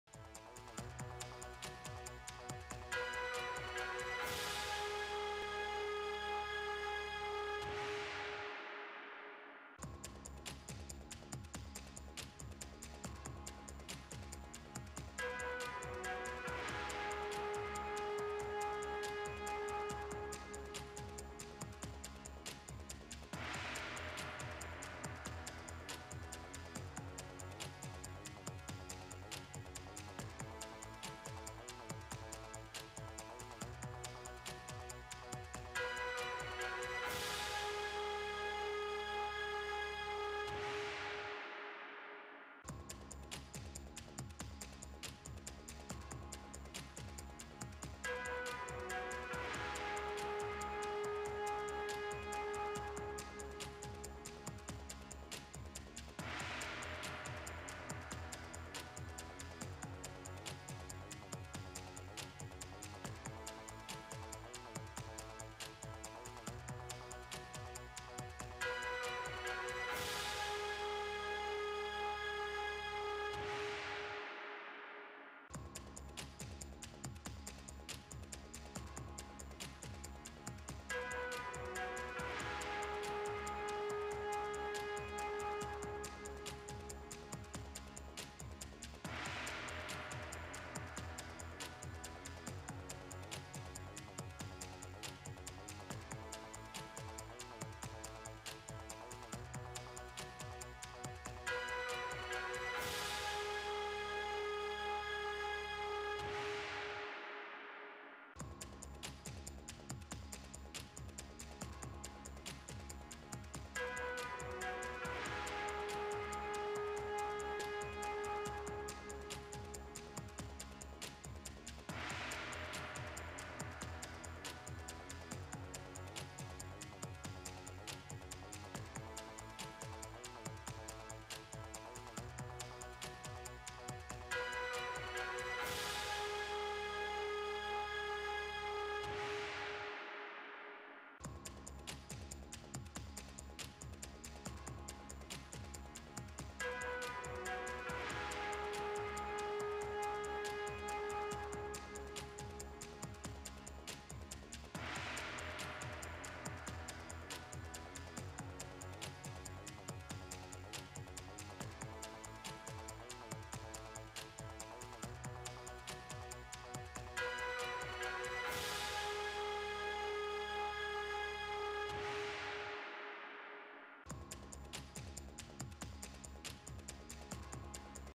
إنفوجراف| كل ما تريد معرفته عن مشروع مدينة الصناعات الغذائية سايلو فودز
3 أغسطس 2021